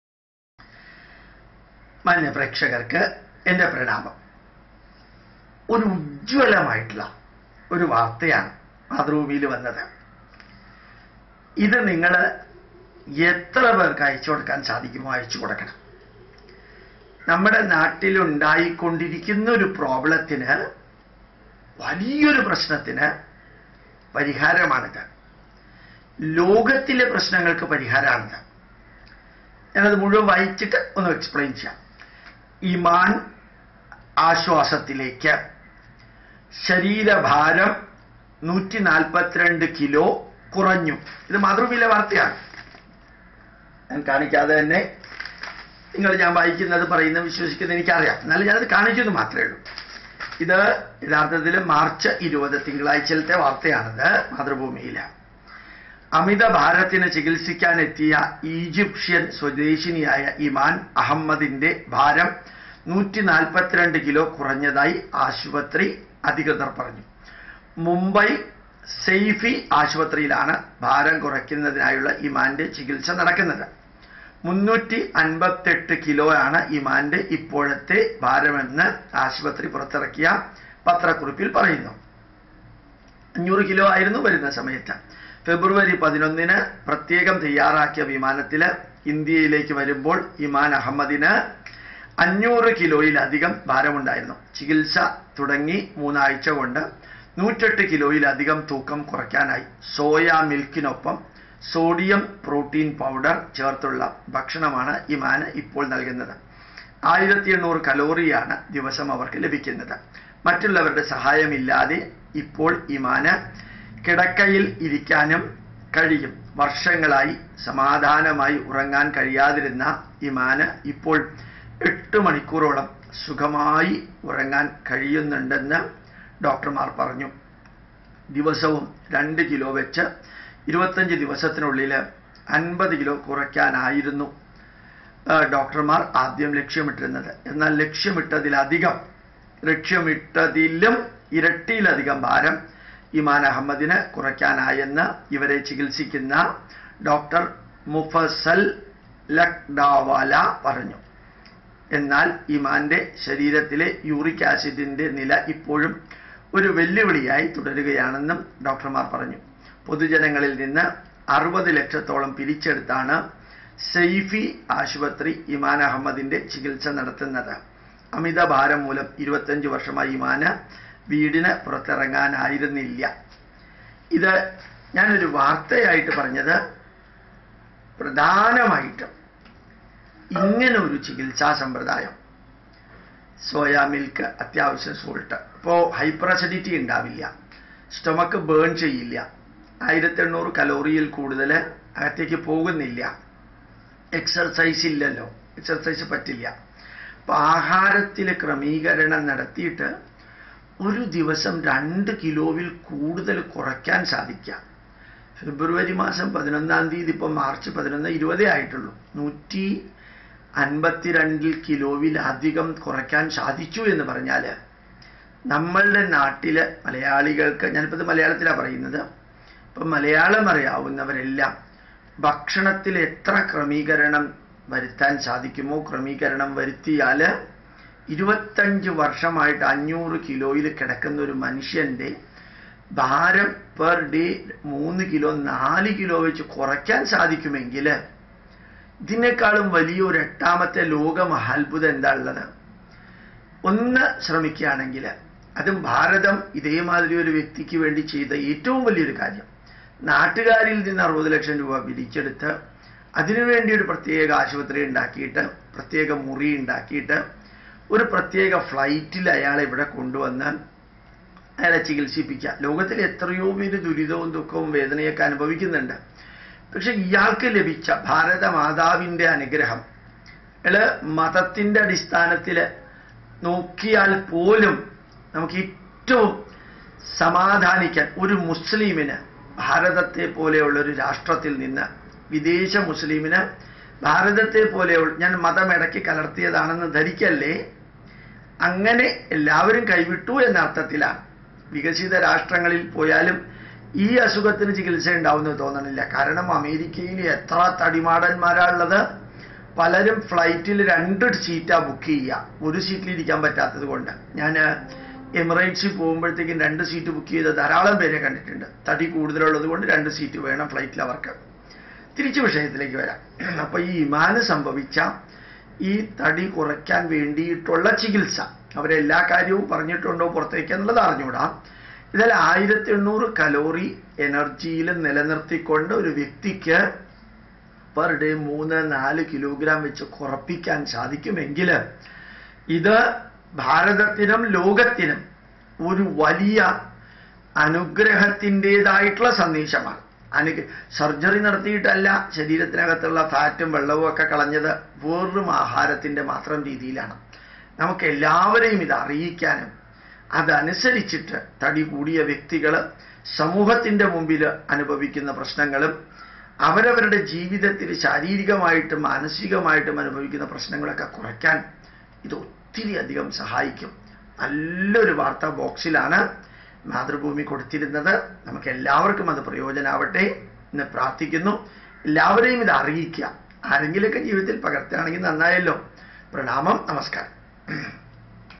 2340- MAL- IMAAN AHAMMADINTE THADI KURAYUNNU- 22 -03- 17
Follow this method for protecting your body from obesity